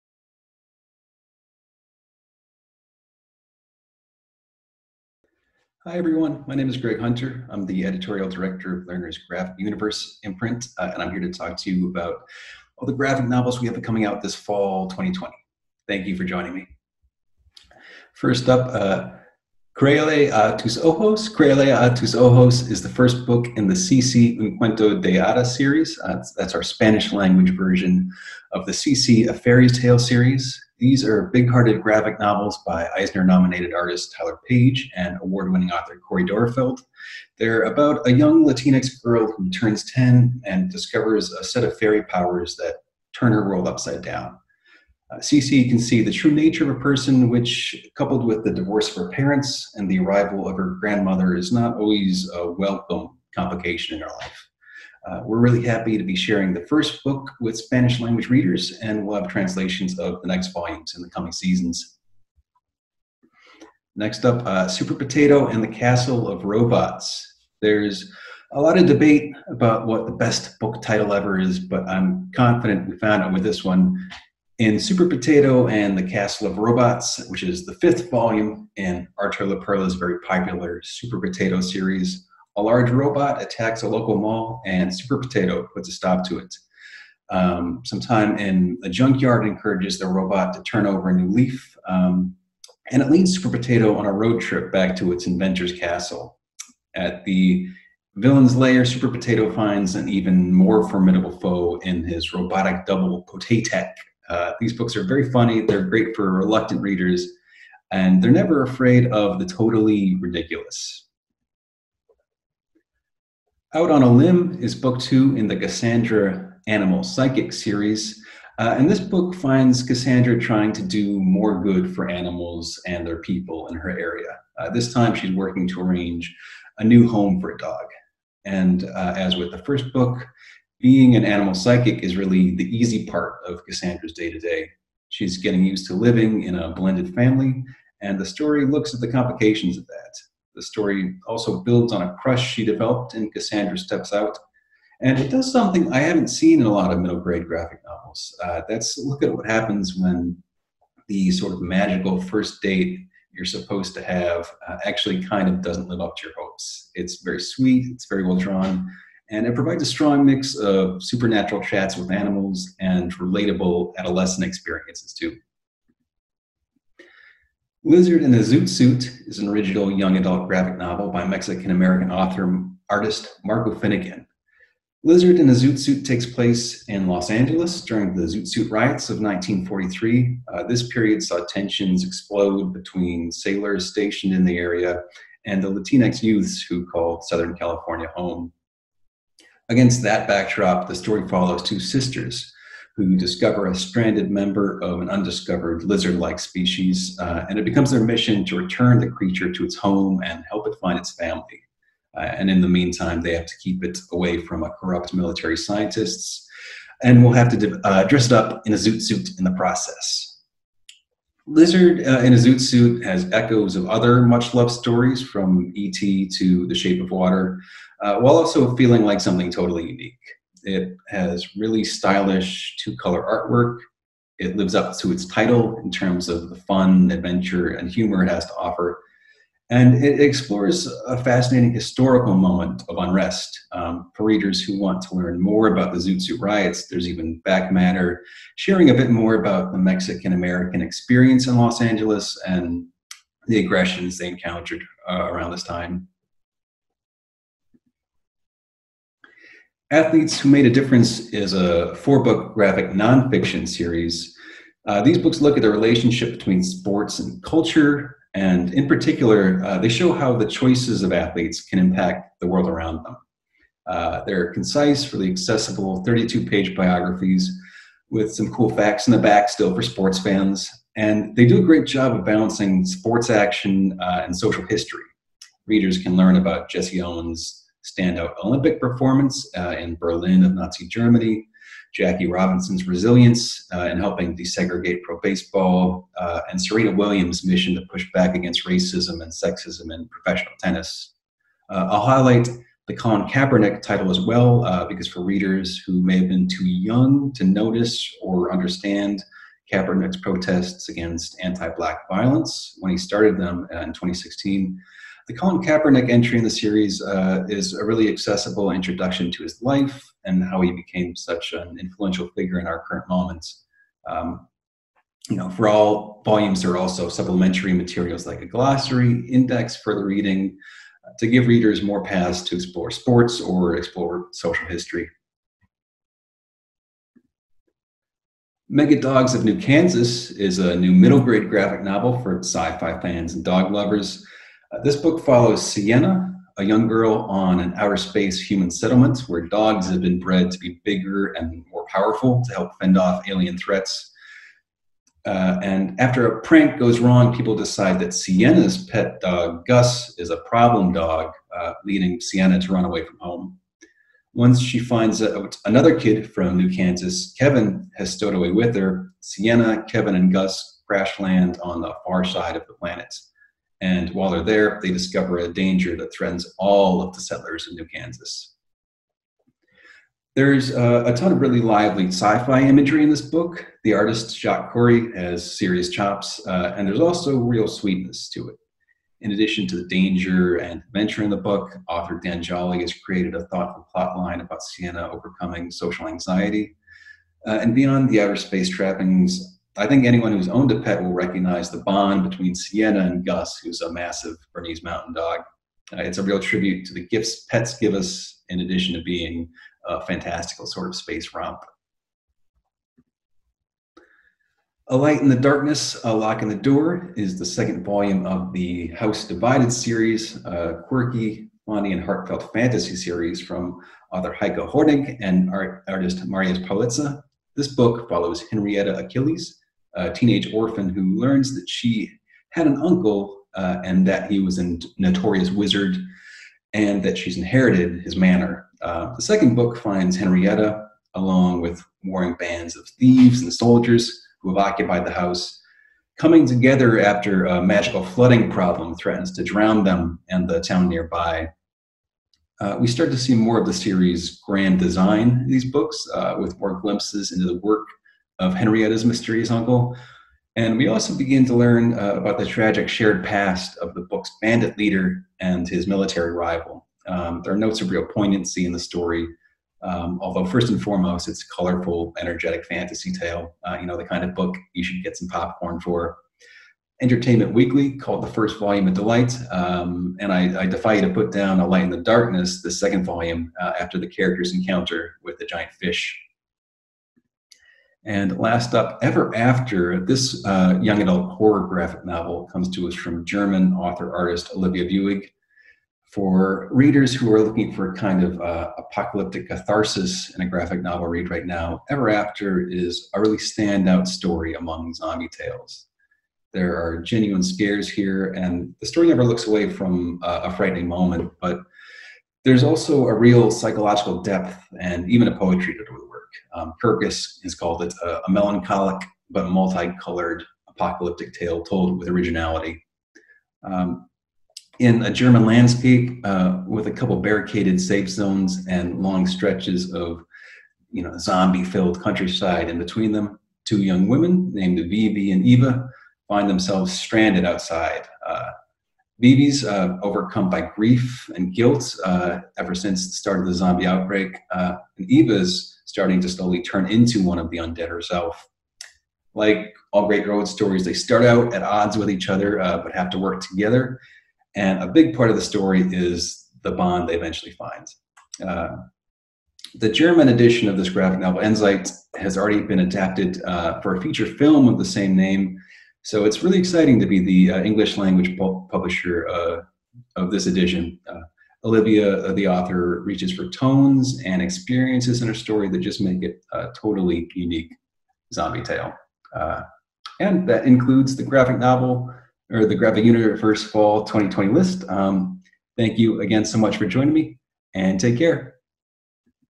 Graphic Universe Fall 2020 Preview
Presented by Greg Hunter, editorial director of Graphic Universe, this sneak peek covers continuations of beloved series like Super Potato, A House Divided, and Cassandra: Animal Psychic, as well as introducing a new Athletes Who Made a Difference series, and titles covering giant extraterrestrial space dogs, a Lizard in a zoot suit, and a gripping zombie tale. See all of Graphic Universe's upcoming titles